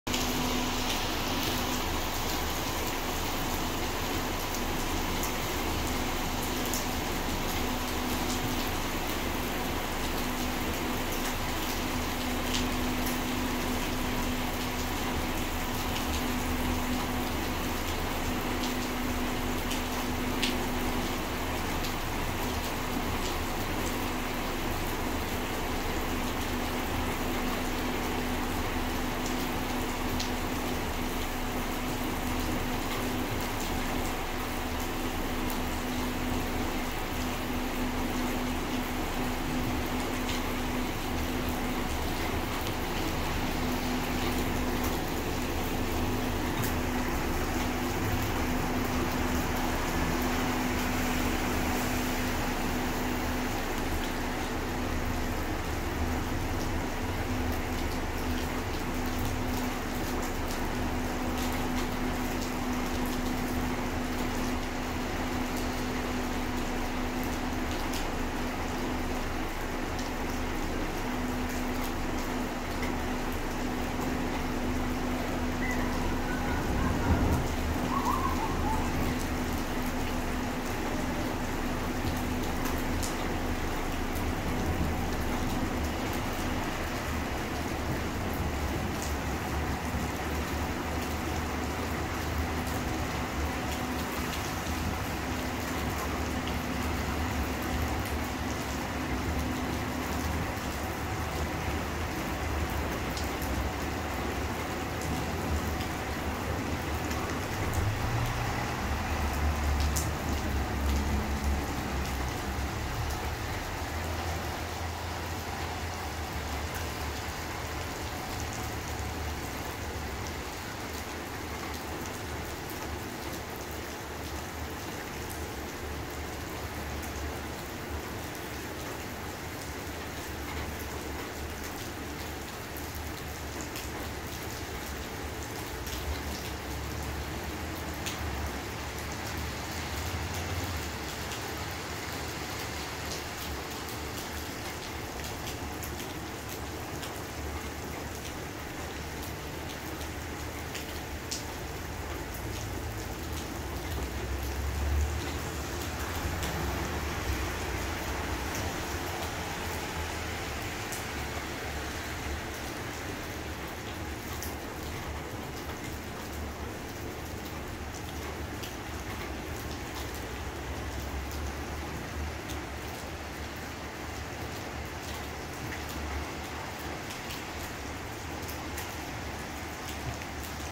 Rainy Night at a Sydney Bus Stop ASMR
Join me for a relaxing evening watching the rain at the metro station. Would you run to avoid the rain?